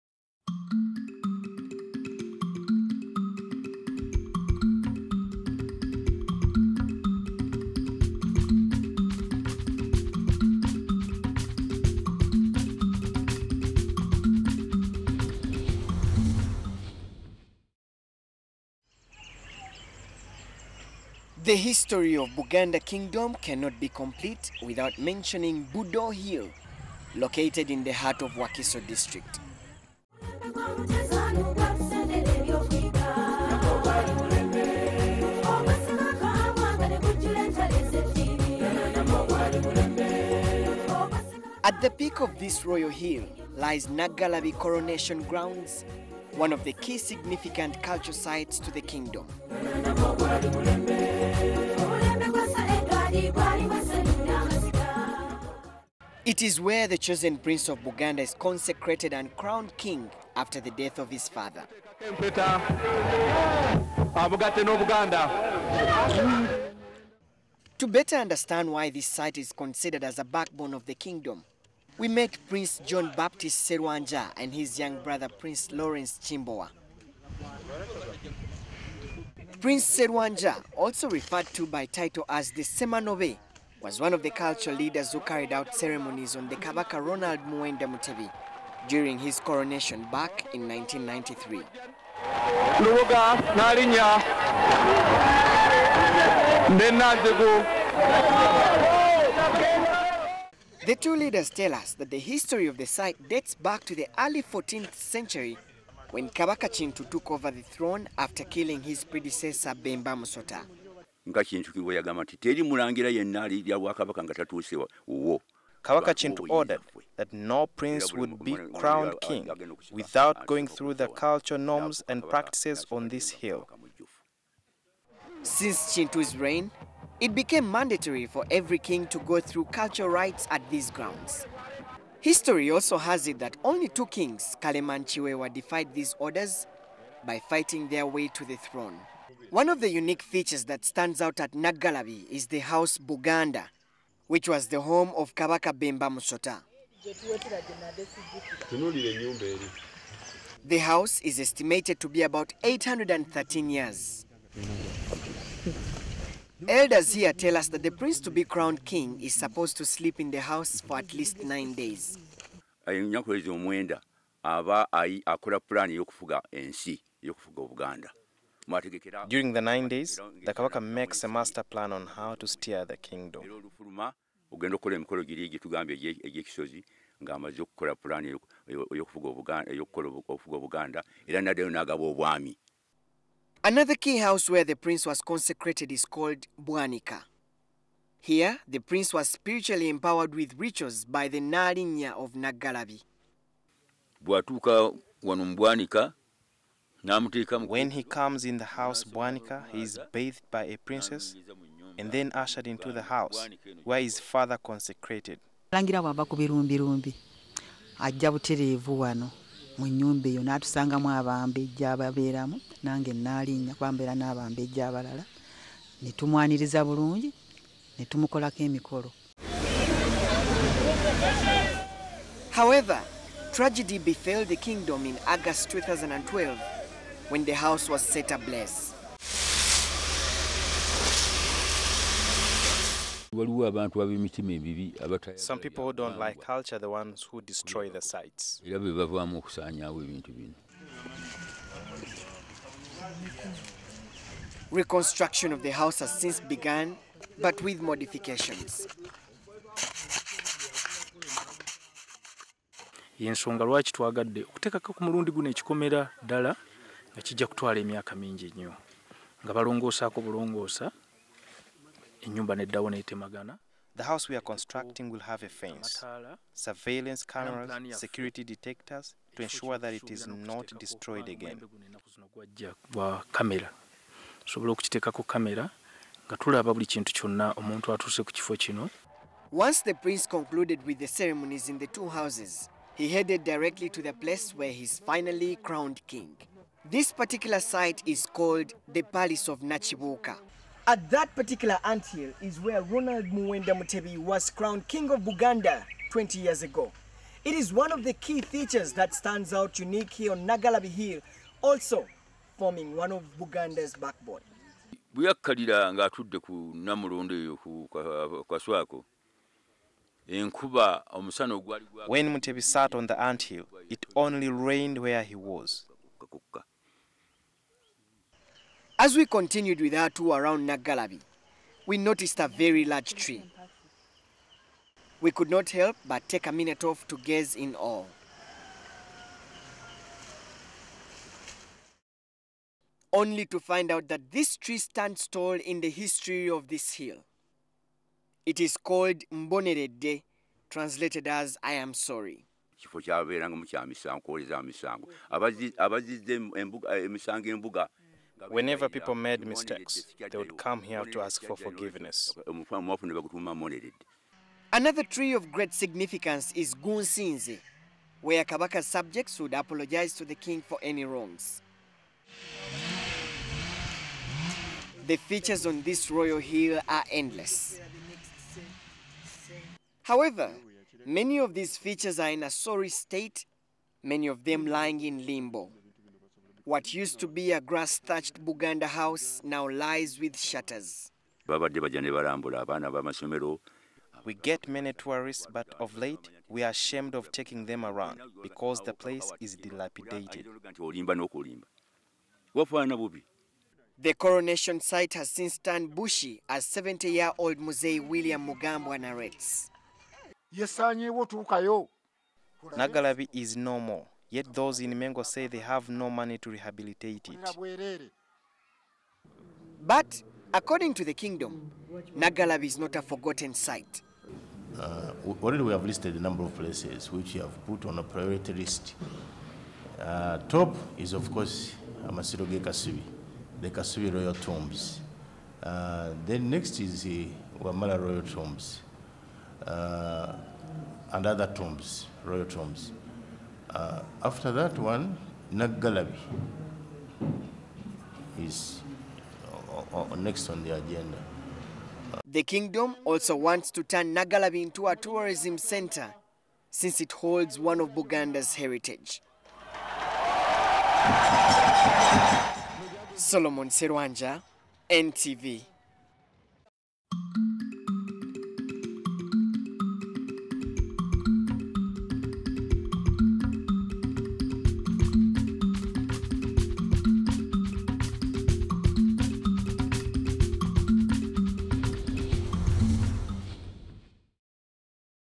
Unravelling the Kabaka Coronation site
In our second feature on Kabaka Muwenda Mutebi's 20th Coronation anniversary.
We focus on Nagalabi cultural site on Buddo hill is one of Buganda's heritage sites. This is where the Kabaka Ronald Muwenda Mutebi was crowned King of Buganda in 1993.